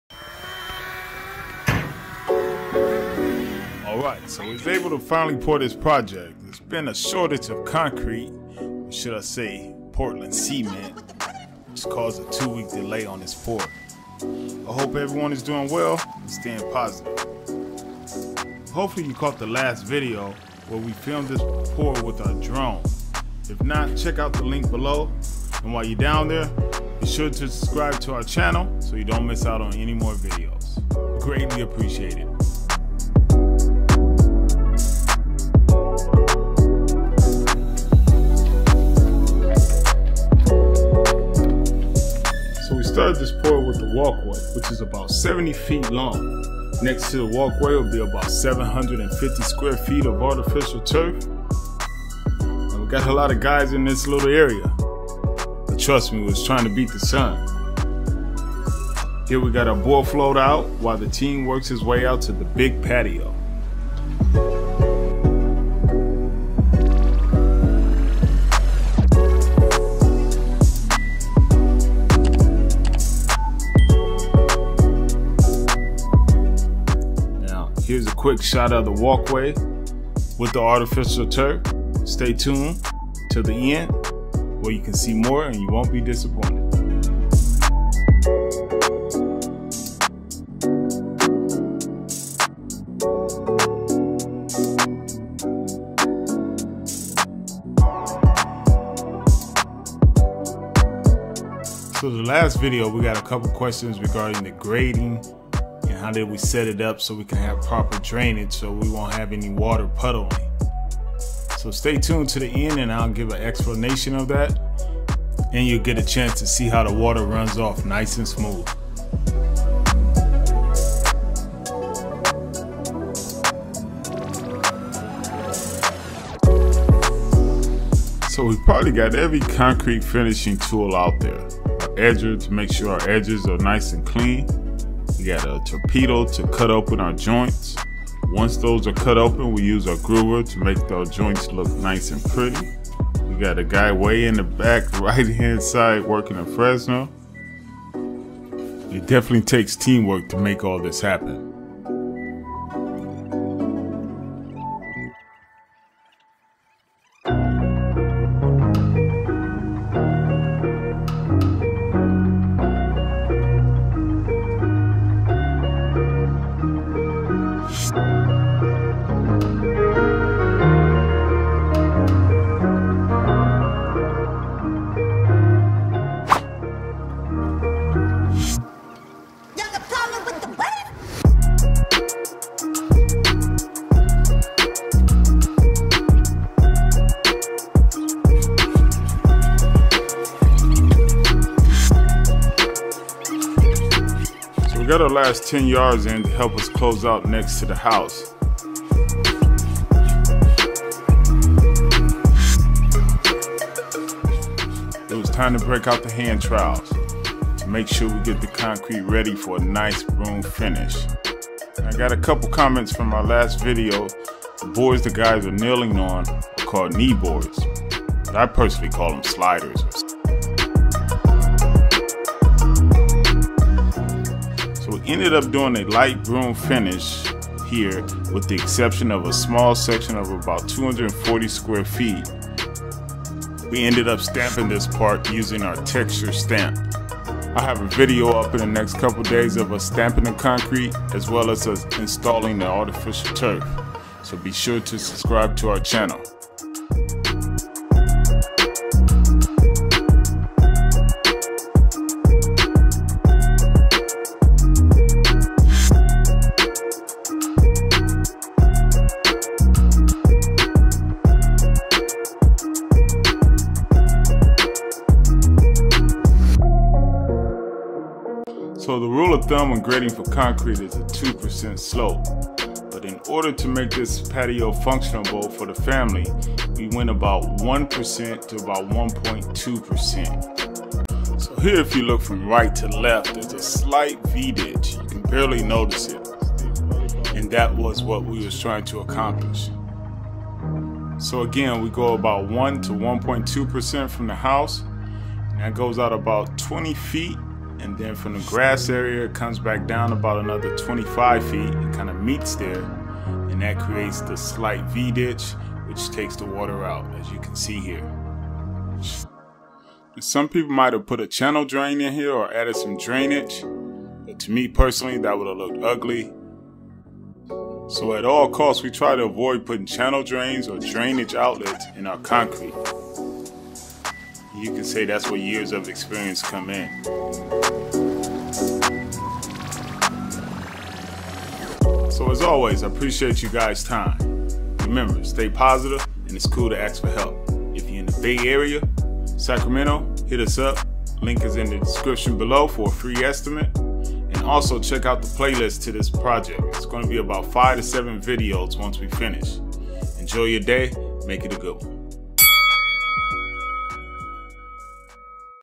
Pouring A Huge Concrete Patio Made Easy!
Check us outpouring this big concrete patio in the backyard. Be sure to watch till the end to see how we set up the grading for proper drainage.